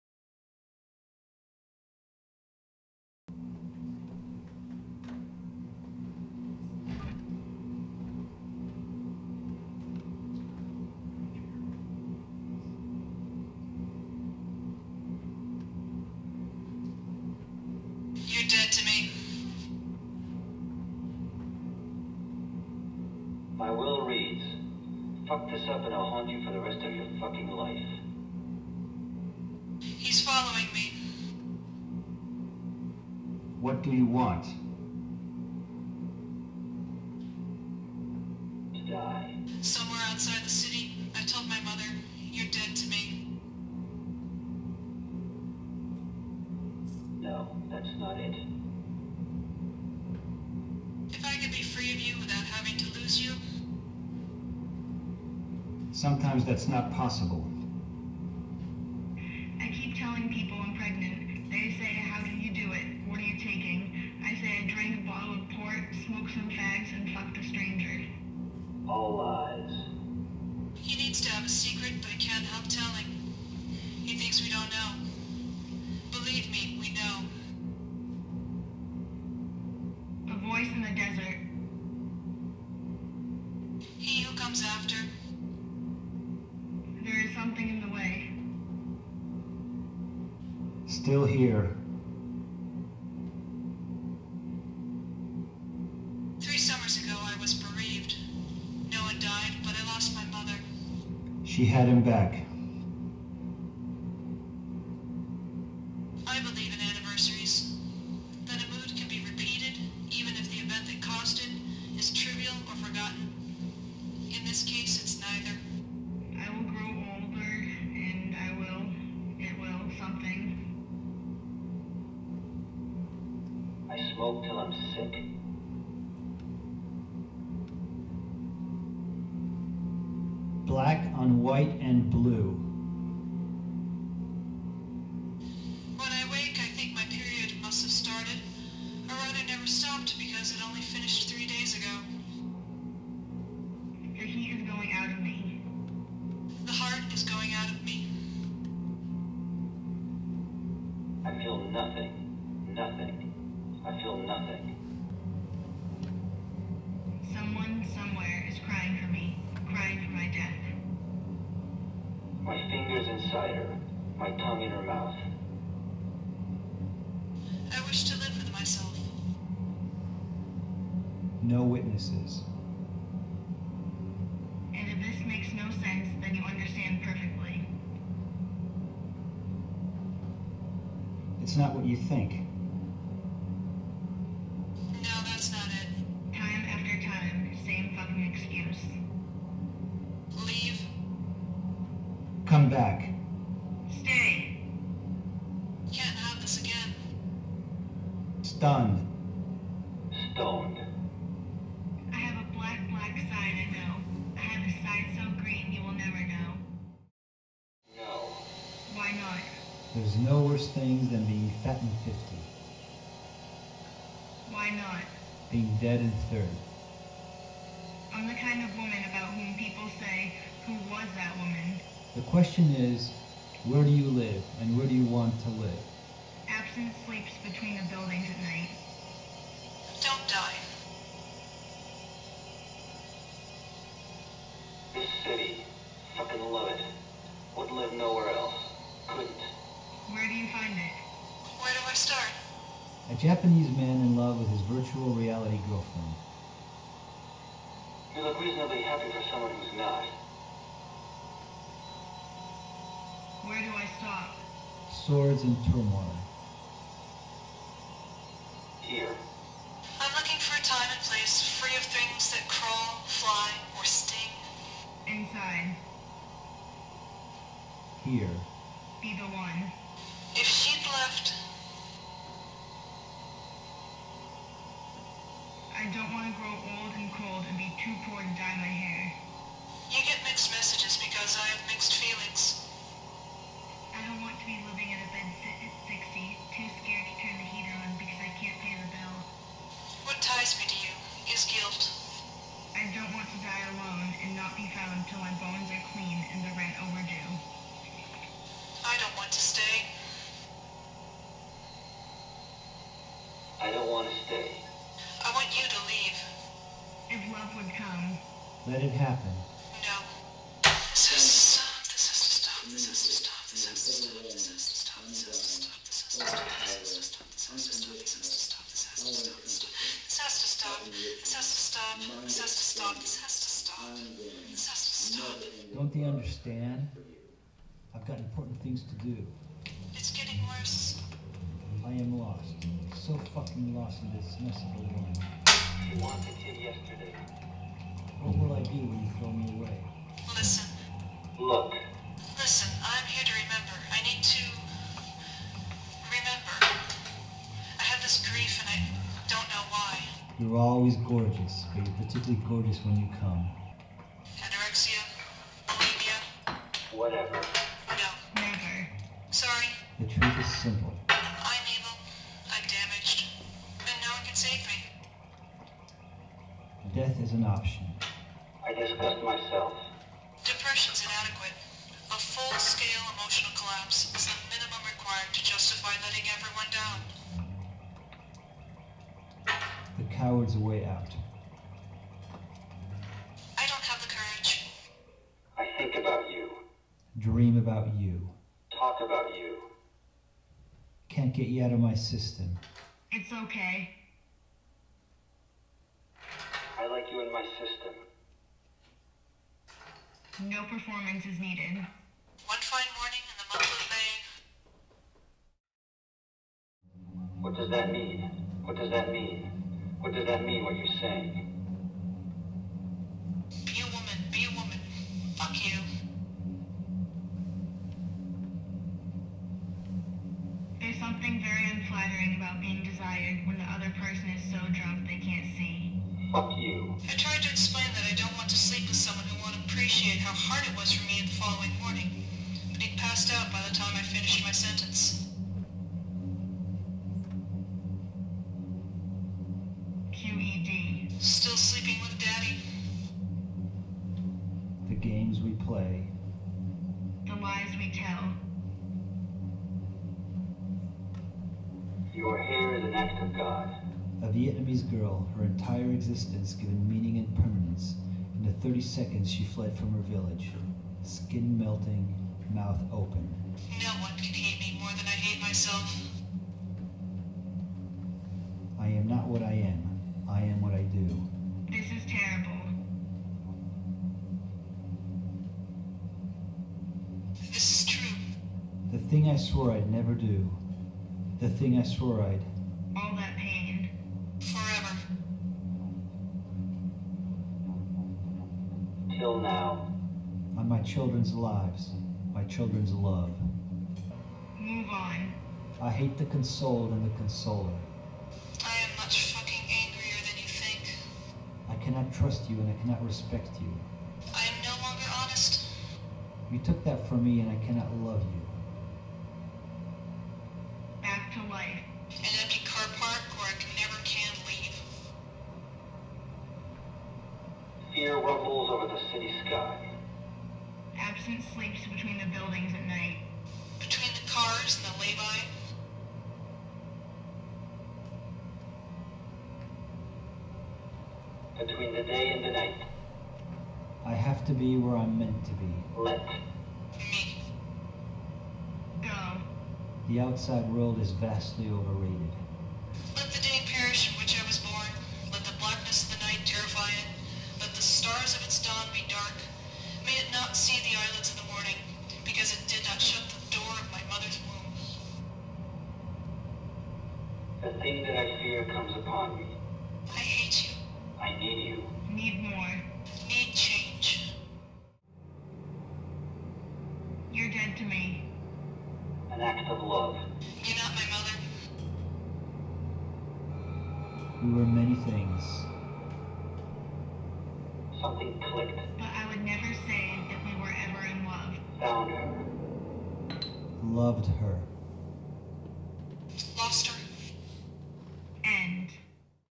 Crave by Sarah Kane. Adapted by Greg Kowalski
Crave by Sarah Kane adapted by Greg Kowalski. Cast: Dei Xhrist, Greg Kowalski, Susan Pepperman. Sound design: Dave Seidel. Interactive video: Greg Kowalski.